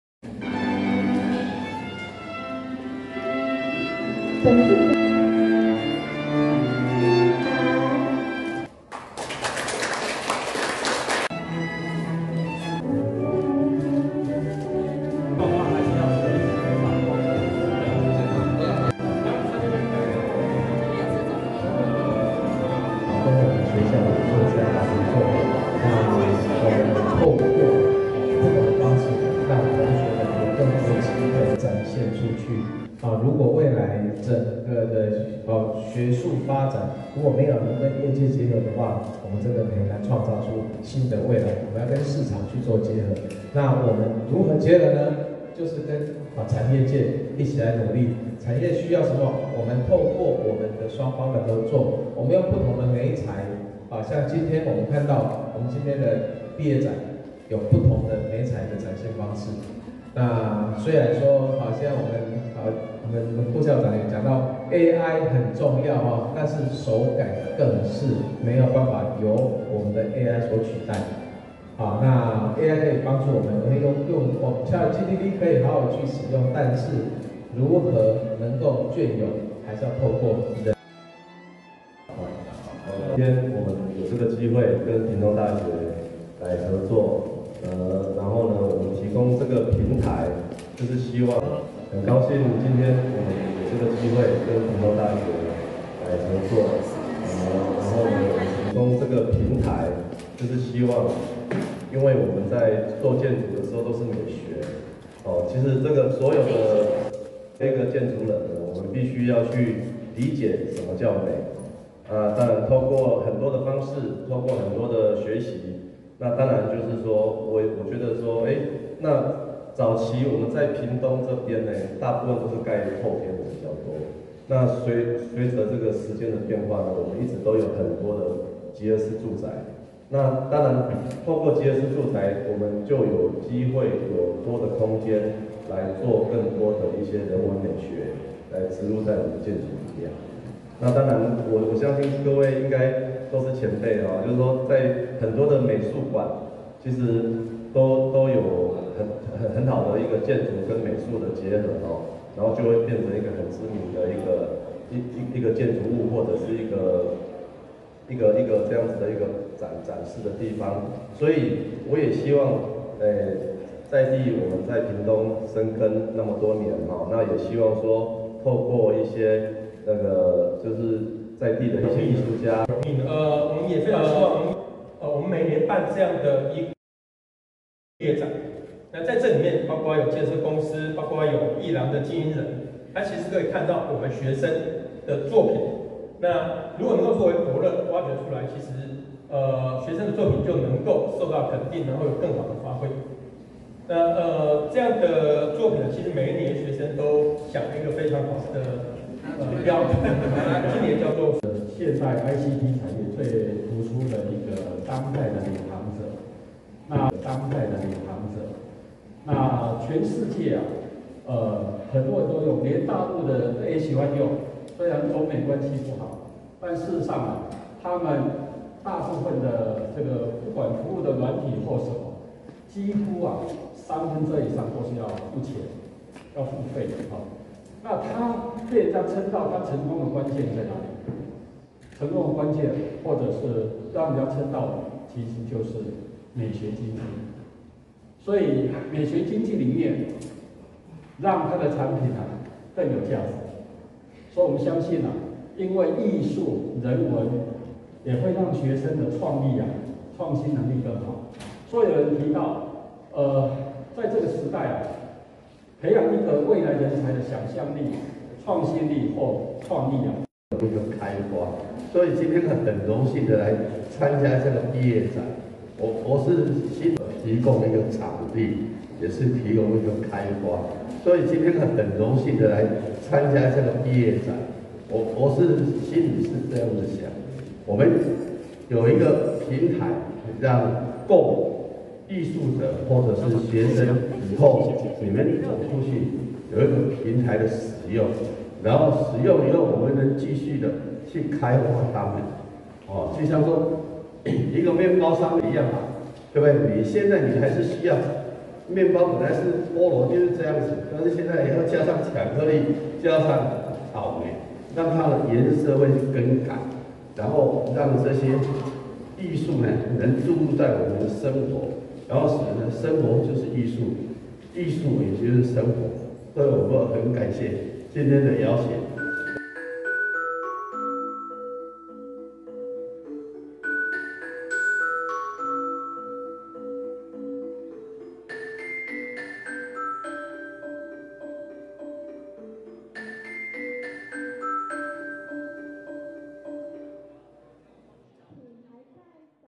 【屏東大學 ・視覺藝術系 】第112級畢業展／與萬吉建設攜手合作／真璞美術／屏東高中
屏大視覺藝術學系112級畢業展

「舒視圈」，從3月13日起，先後在#屏大屏師校區六愛樓一樓藝文中心、青春美展高雄文化中心至真堂一館、青春設計節高雄駁二蓬萊B4倉庫、放視大賞高雄展覽館南館等地方，進行巡迴展出。

主題「舒視圈」，不僅是要跳脱舒適圈，而且要跨大舒「視」圈。學生既能理性亦能感性，隨靈感創造出與眾不同的作品，展示於眾人眼前。

開幕式中，視藝系也與在地的萬吉建設簽署「萬吉閱山藝術創作」產學合作計畫案。

萬吉建設 致力於土地開發、建築設計、景觀規劃、室內空間設計、企業文創商品規劃、在地藝文公益贊助等，賴彥良董事長思考如賦予建築「人文」，並尋找各種支持藝術家的方法，期盼能把「人文」移植在建築裡面，歷年建案中公共空間設計，都會規畫放置屬於這個案場精神的藝術品。

這項產學合作計畫，由視藝系劉懷幃老師主責，將以萬吉建設機構旗下建案「萬吉閱山」為創作主軸，由視藝系師生進行藝術創作。

未來屏大與萬吉建設，共同推動藝術人才培育的工作，持續提供學生藝術創作的平台與機會，讓藝術與建築完美結合，共創雙贏。
視藝系畢展巡迴展如下，歡迎對藝術創作與數位媒體設計有興趣的朋友共襄盛舉。

◎校內展：3月13日~3月24日
◎青春美展：4月14日~4月25日
◎青春設計節：5月11日~5月14日
◎線上展：5月12日開始
◎放視大賞：5月25日~5月27日